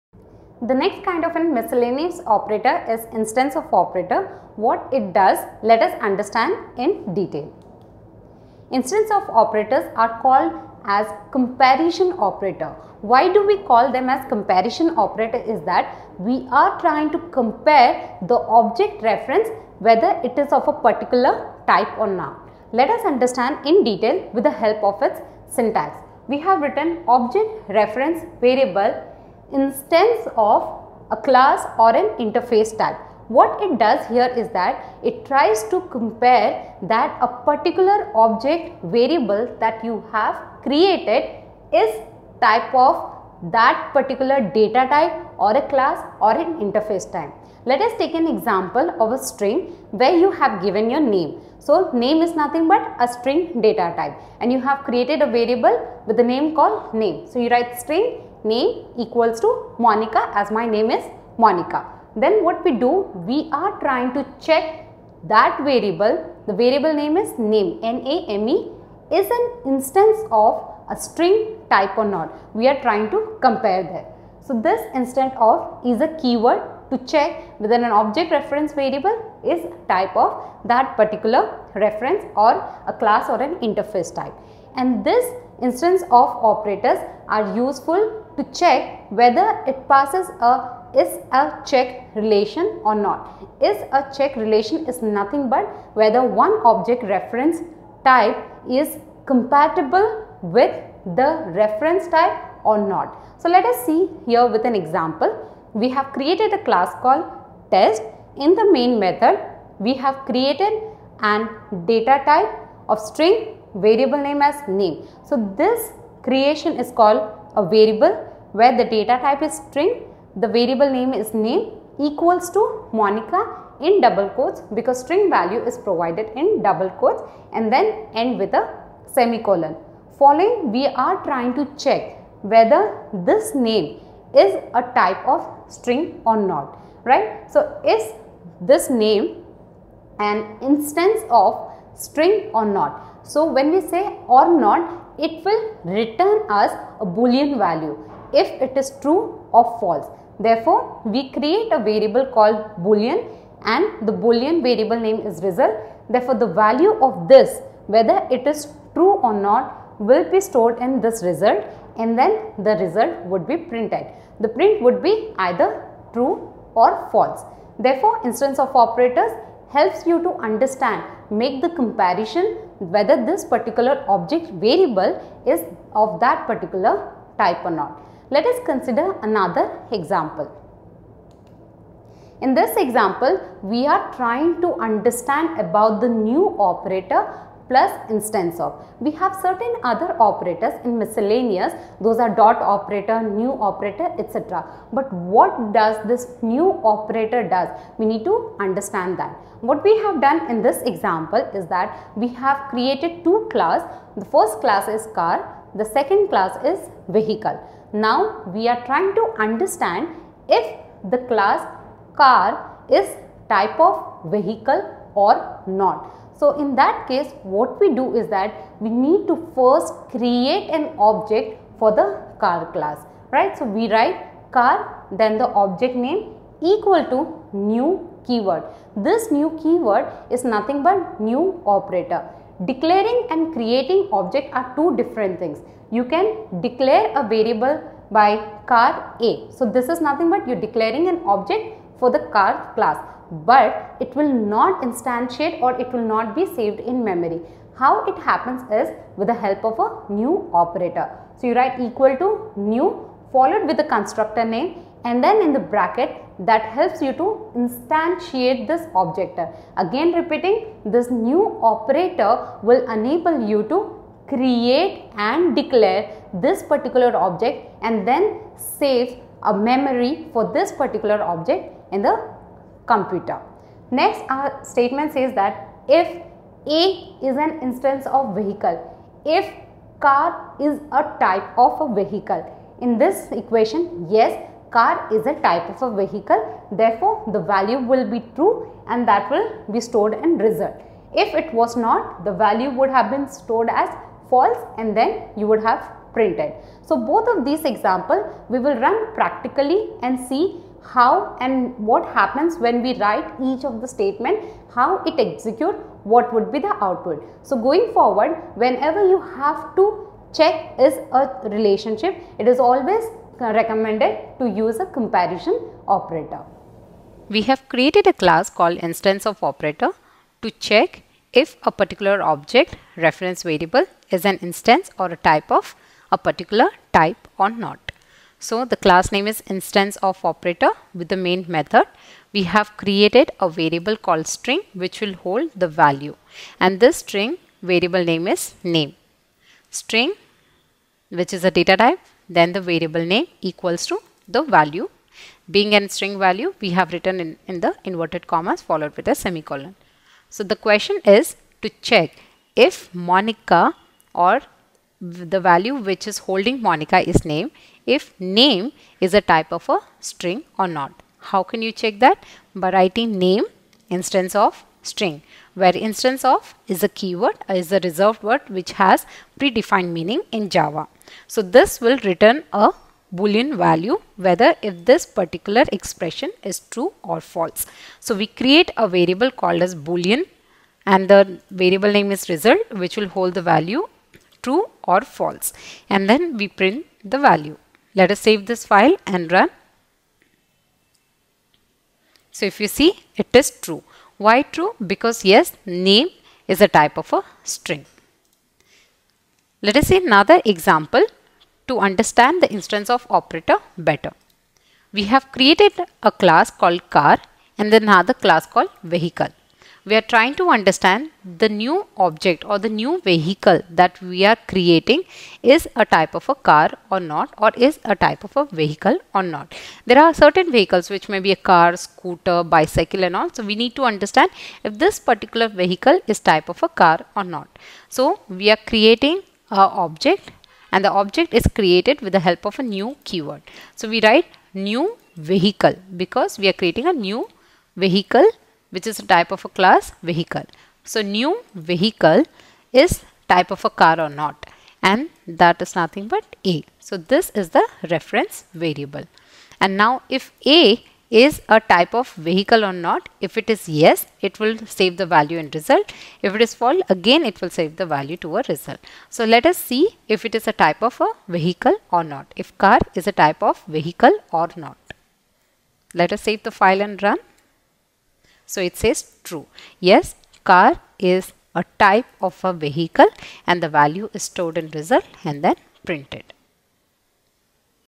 Java - Instance of Operators
Lecture By: Ms. Monica, Tutorials Point India Private Limited